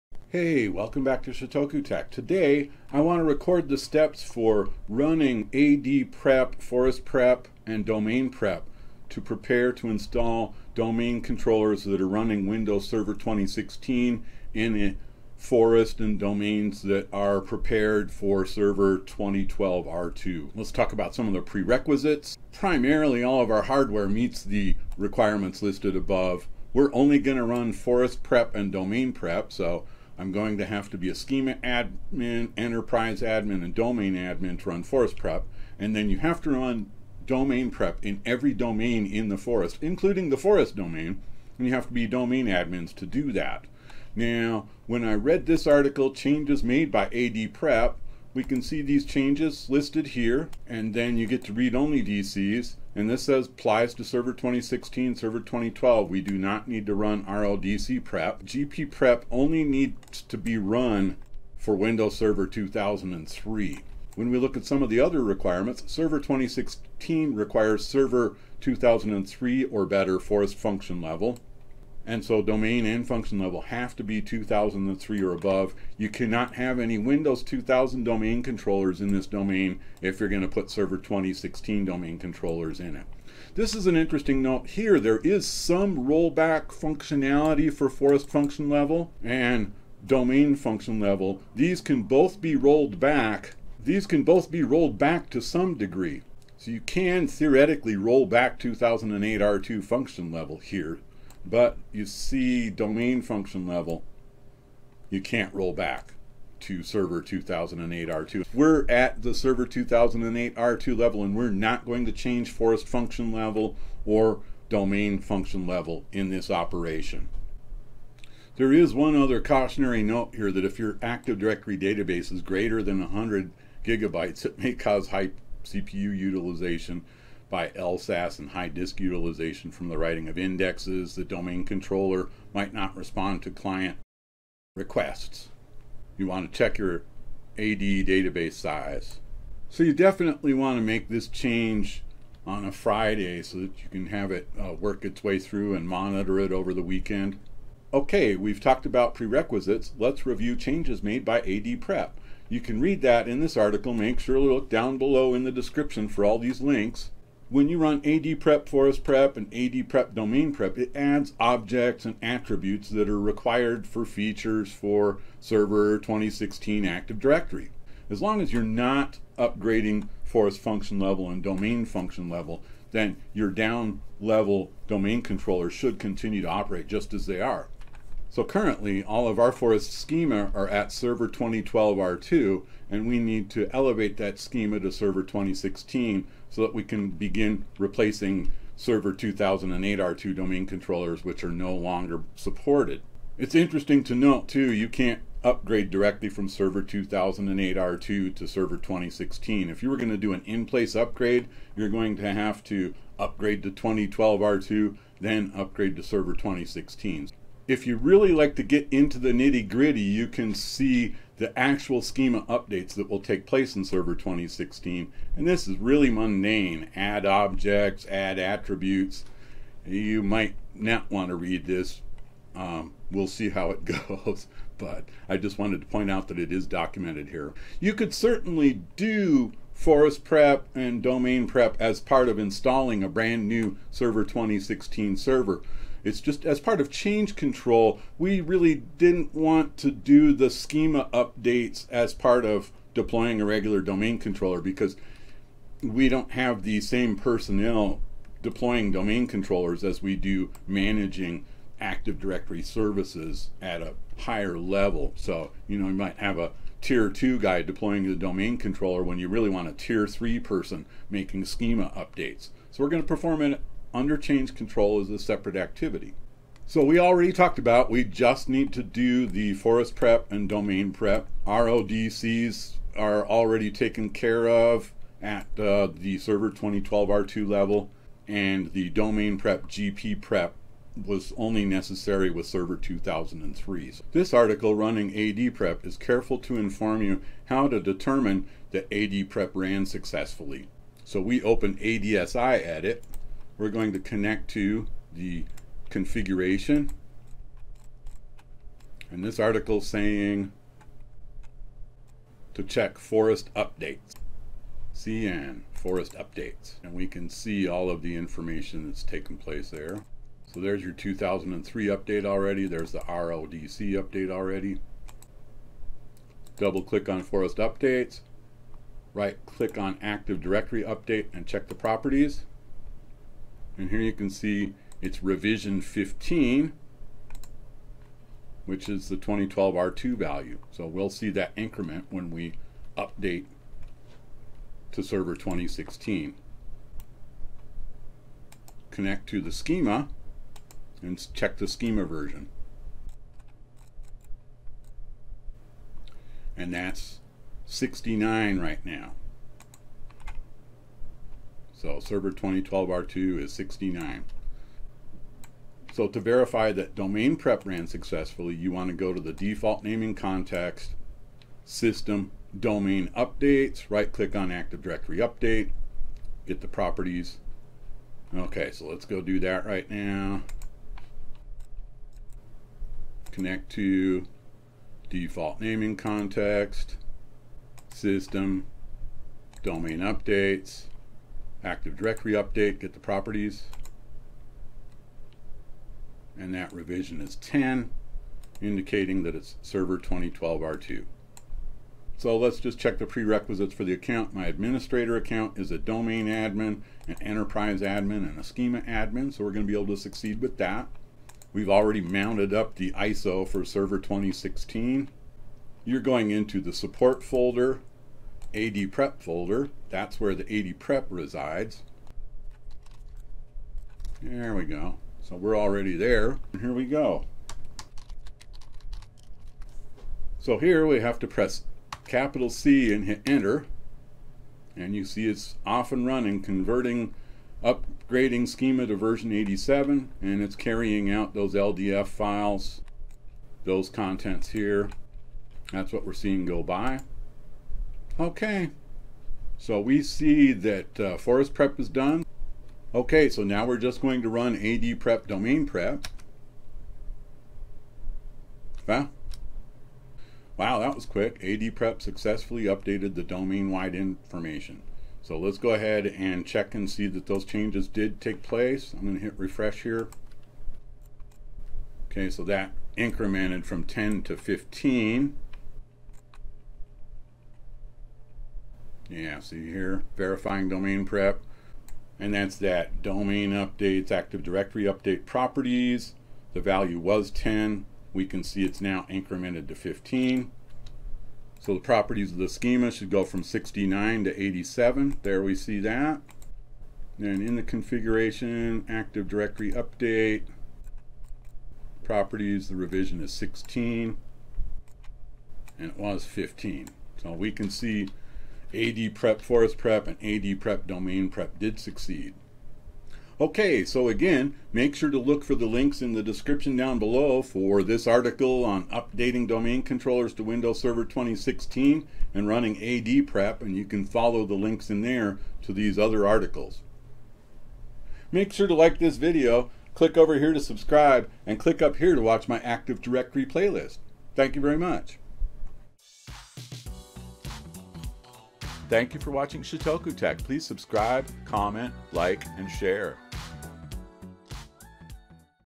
ADPREP Command Complete Tutorial Server 2016
Learn how to run ADPREP to prepare Server 2012 R2 Active Directory Forest and Domains for Server 2016 Domain controllers. The prerequisites, the changes to the Schema and how to verify successful completion. It is interesting to not that Forest Function Level and Domain Function Level do have some rollback capability.